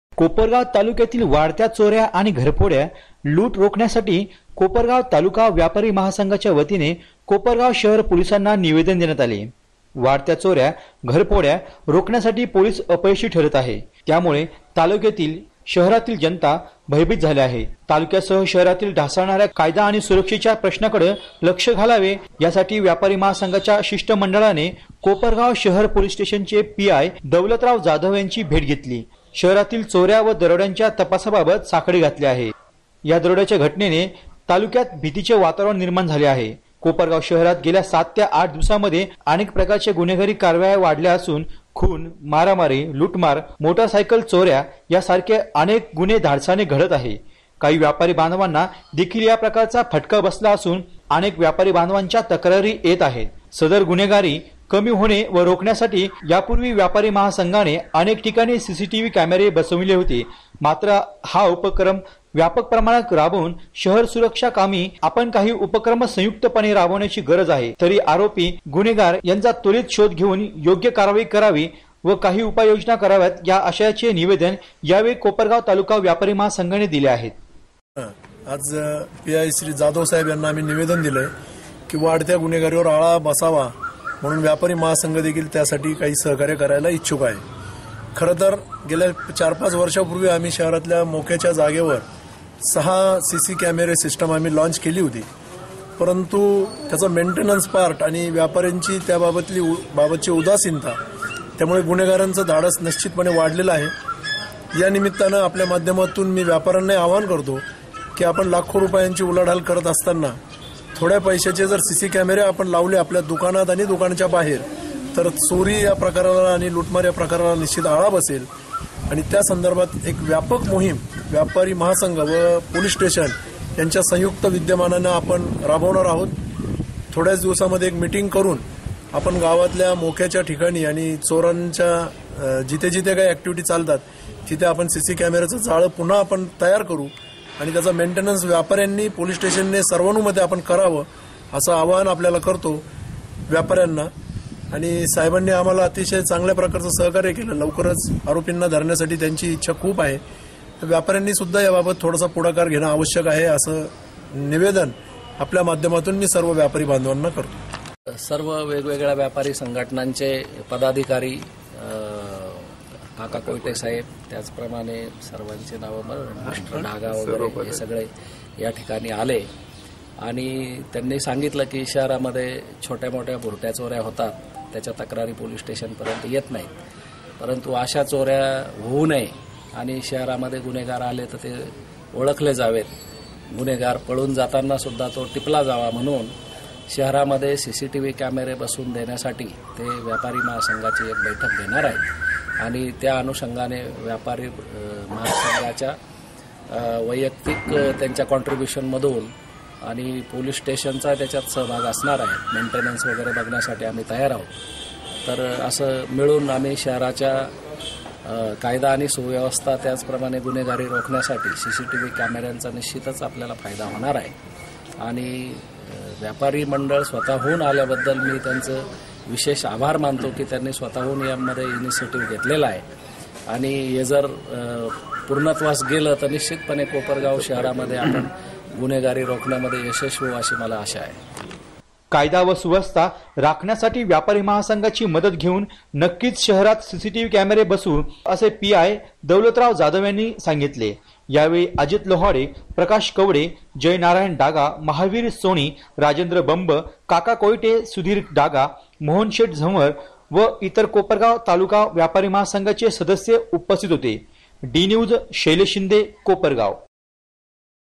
कोपरगाव,वाढत्या चोर्या रोखण्यासाठी व्यापारी महासंघाचे निवेदन
शैलेश शिंदे ( डी न्यूज़ )